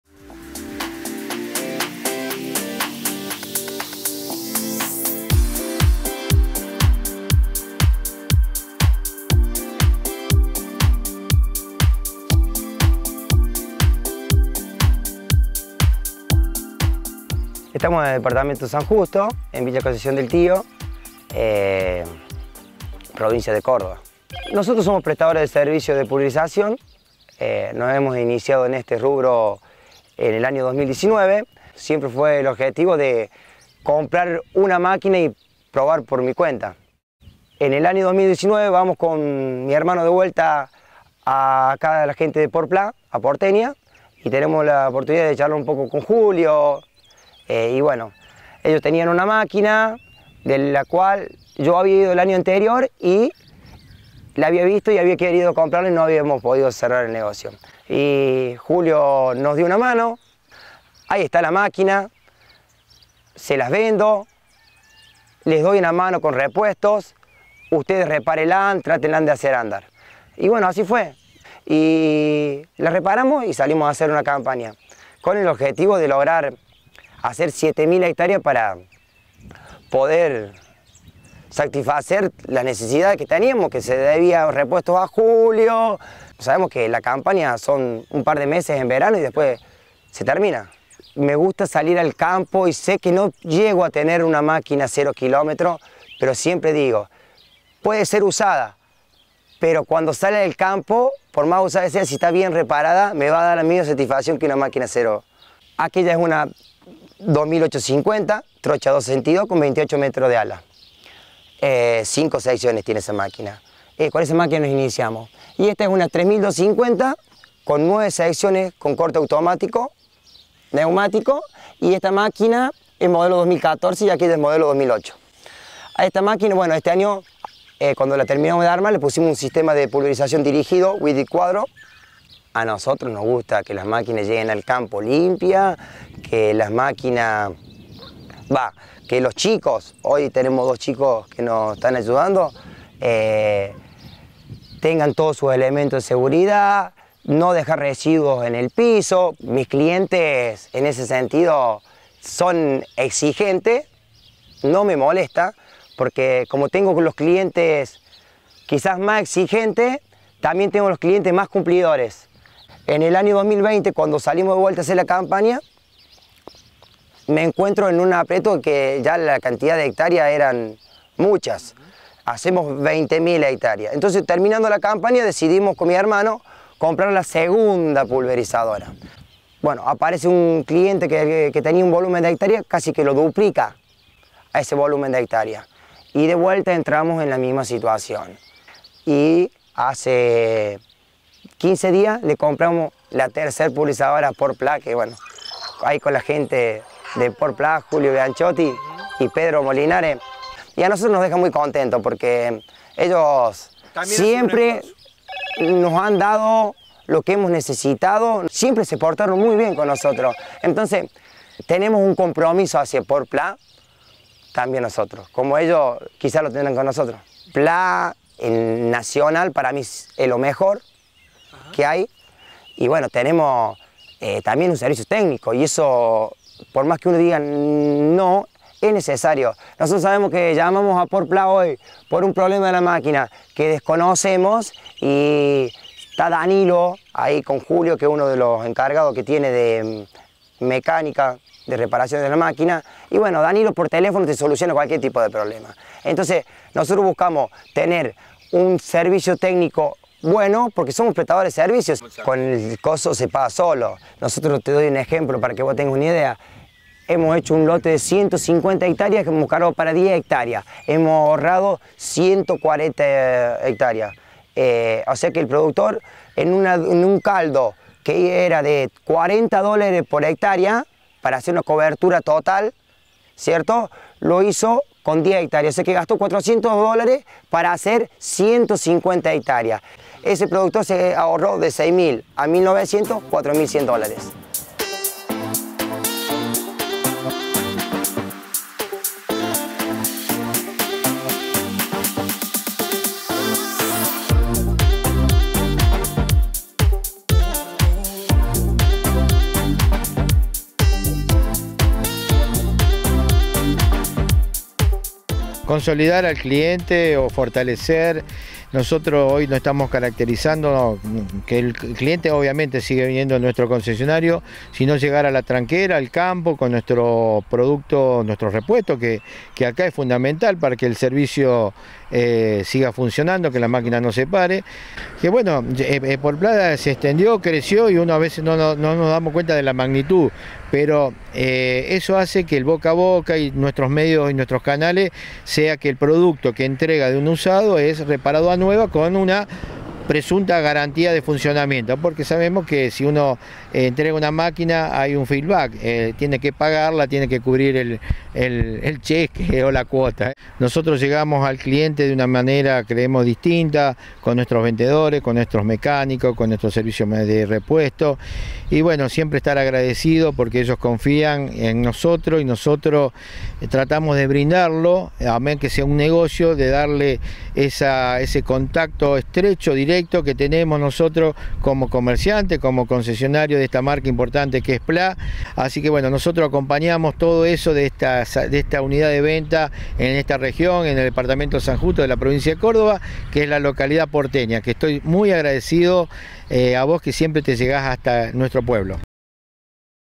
Los usuarios de usados Port Pla más que satisfechos
Pulverizadoras usadas como nuevas, una interesante estrategia de los clientes Port Plá, para crecer prestando un  servicio de calidad.